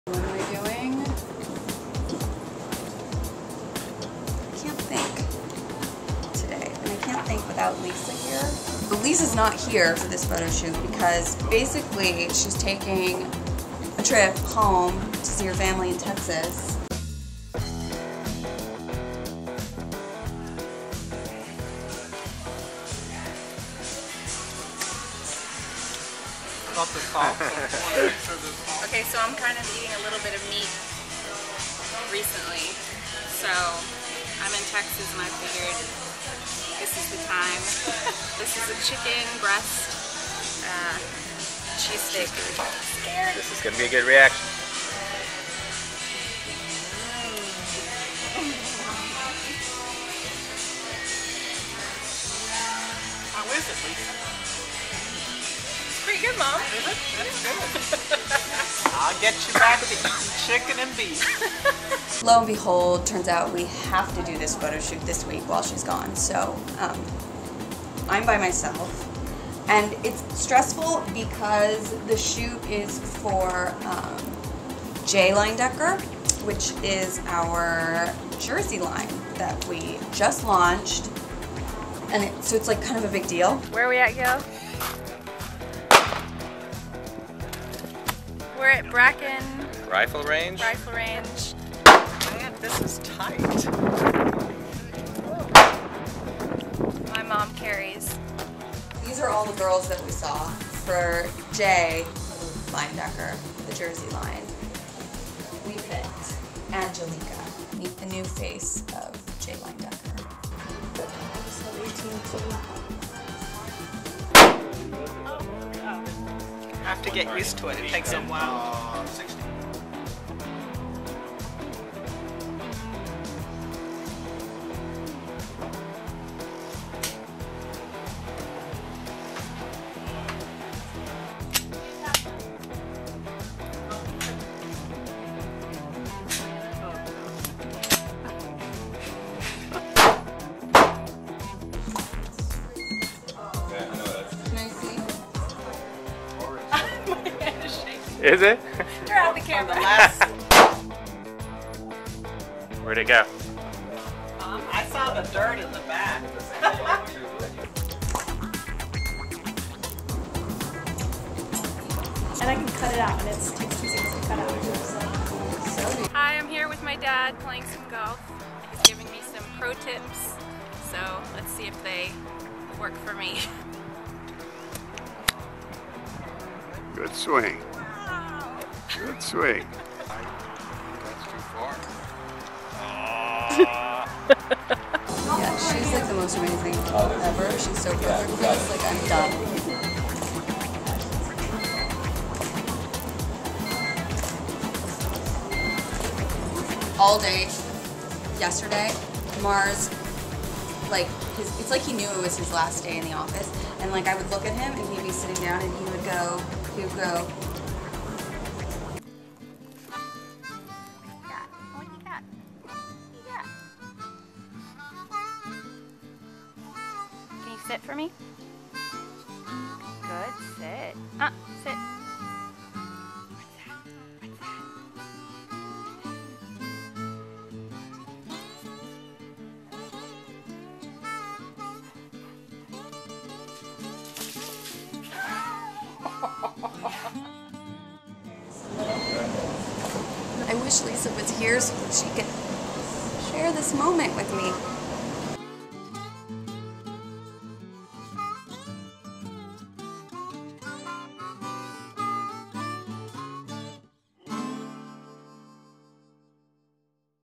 Guns and Glamour!
Separation anxiety! Lisa goes back to her roots with a trip home to Texas while Jessica is left in charge back in LA.  Photoshoots and shooting ranges? We do it all at Leyendecker. Guns and glamour, baby!

Executive Producer: Jenny Fancy